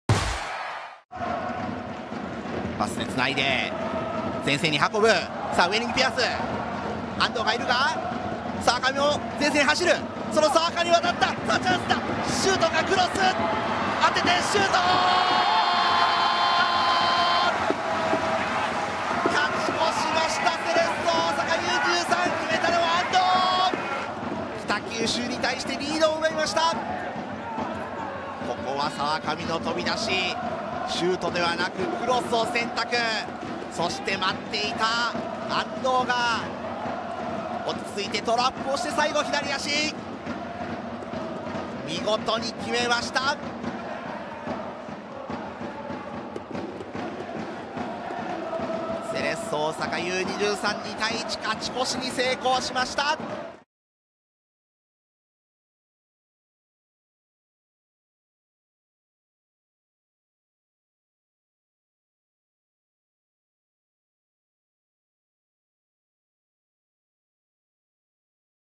【公式】ゴール動画：安藤 瑞季（Ｃ大23）85分 セレッソ大阪Ｕ-２３vsギラヴァンツ北九州 明治安田生命Ｊ３リーグ 第16節 2019/7/14
2019年7月14日（日）に行われた明治安田生命Ｊ３リーグ 第16節 C大23vs北九州の安藤 瑞季（Ｃ大23）のゴール動画です。